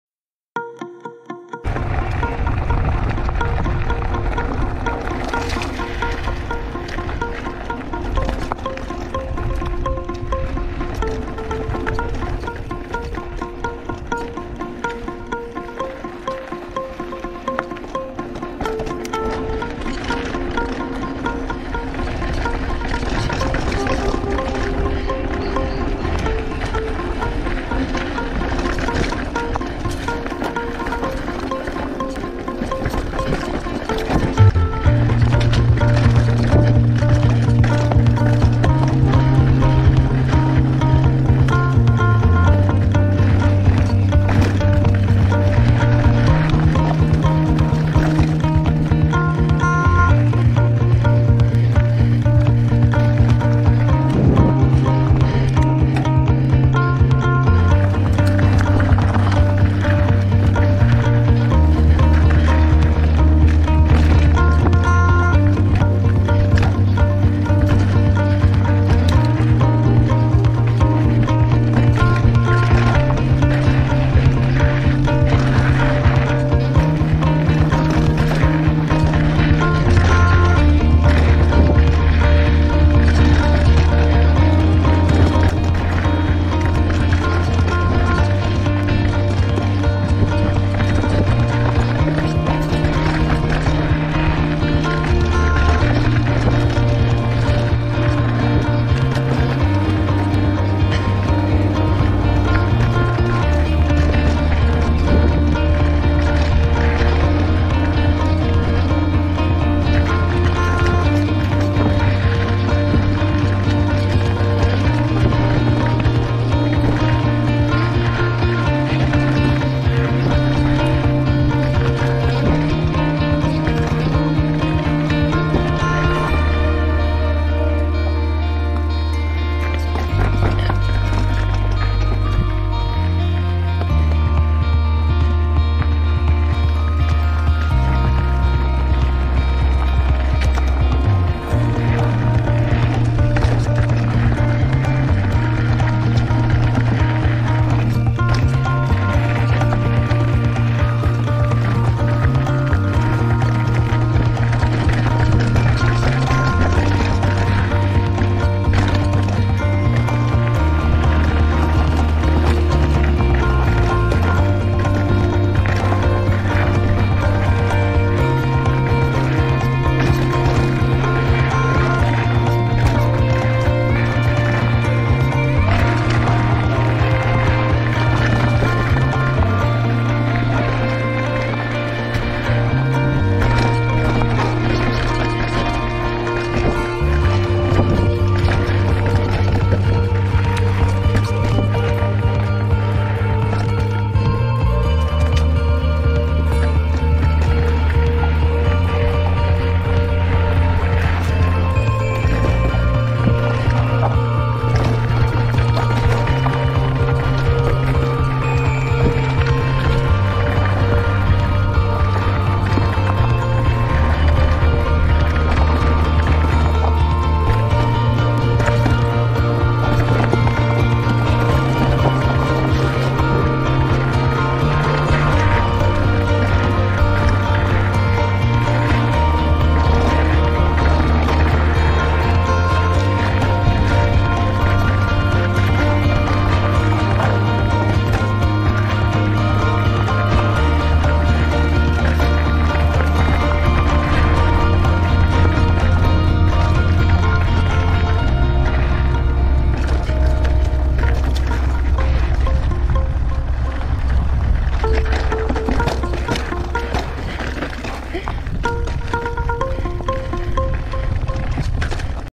Woolshed Falls 2022 - Beechworth MTB Downhill Trail
🍃💫 Woolshed MTB Trail 💫🍃

Last time I did this trail was almost 2 years ago and there were 3 small spots, I didn't have the confidence to do last time.

After having COVID last week, thought it was a good idea to go to a quiet trail.

I was pleased with myself that my skill level has definitely improved and I can do this whole trail now.

I'm still not perfect with corners and jumps but I'll get there one day.